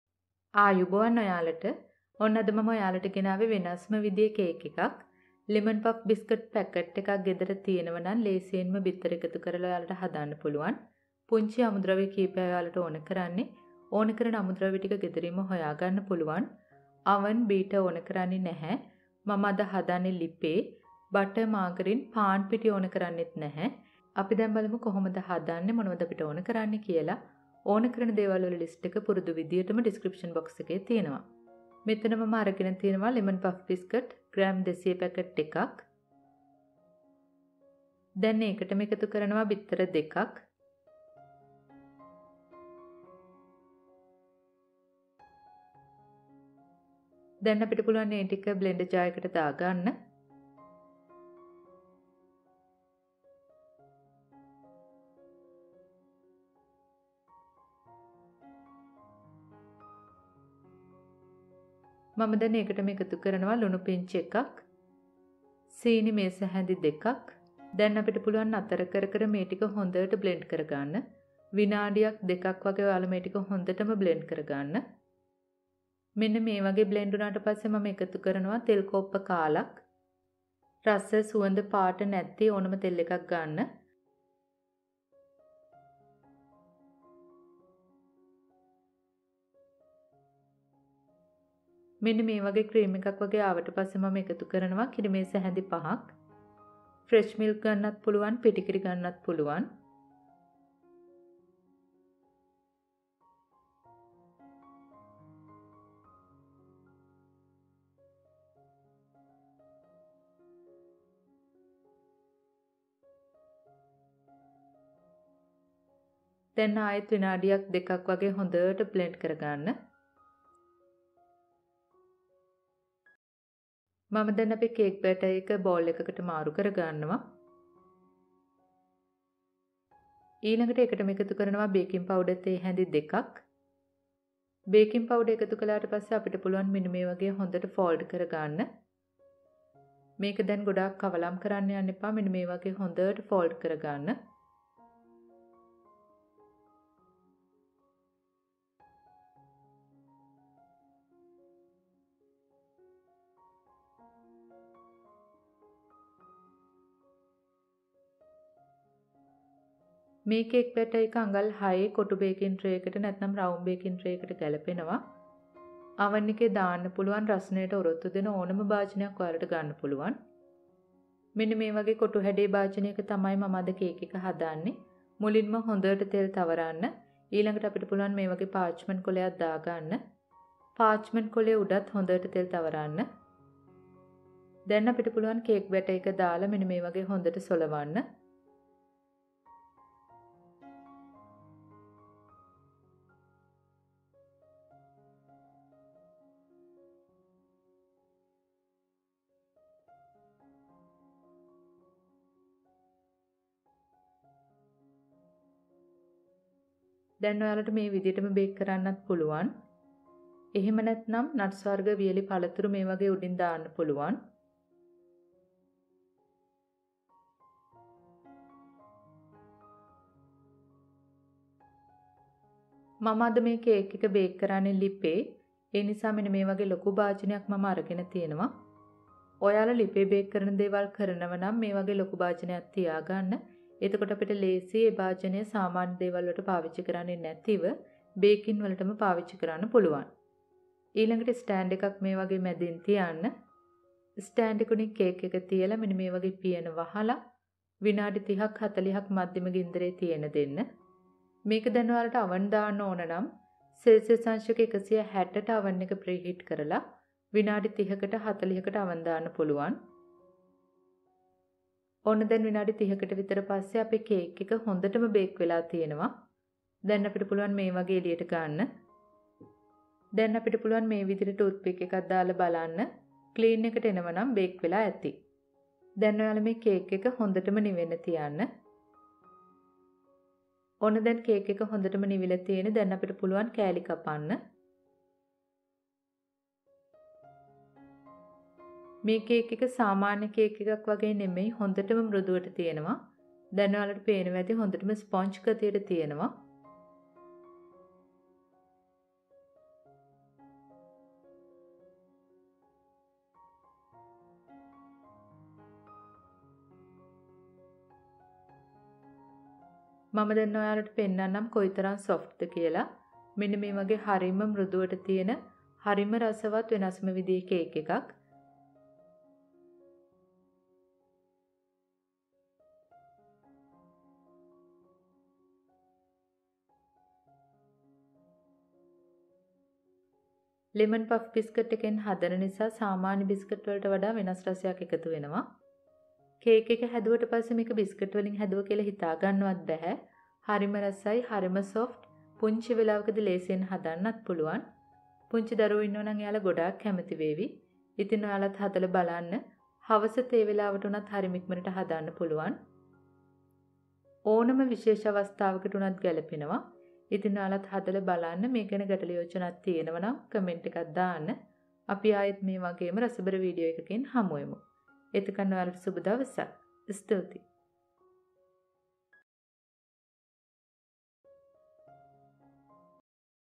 ✔️ ලිපේ හදන පිටි, බටර්, මාගරින් මොනවත් නැති මැජික් අතුරුපස ❤ Magic Biscuits Dessert | Chammi Imalka
✔️ ලිපේ හදන පිටි, බටර්, මාගරින් මොනවත් නැති මැජික් අතුරුපස ❤ Magic Biscuits Dessert
Ingredients
1. 200g of Lemon puff biscuits
2. 2 Eggs
3. A pinch of Salt
4. 2 tbsp of Sugar
5. 1/4 cup of Oil (60 ml)
6. 5 tbsp Milk powder
7. 2 tsp of Baking powder

Optional
8. A drop of Yellow food coloring
9. Some Dry fruits &Nuts

1. ලෙමන් පෆ් බිස්කට් ග්‍රෑ 200
2. බිත්තර 2
3. ලුනු පිංචක්
4. සීනි මේසහැදි 2
5. තෙල් කෝප්ප 1/4 (මි ලී 60)
6. කිරි මේසහැදි 5
7. බේකින් පව්ඩර් තේහැදි 2

අමතර දේවල්
8. කහ පාට ෆුඩ් කලරින් බිංදුවක් (අත්‍යාවශ්‍ය නෑ)

9. වියළි පලතුරු හා නට්ස්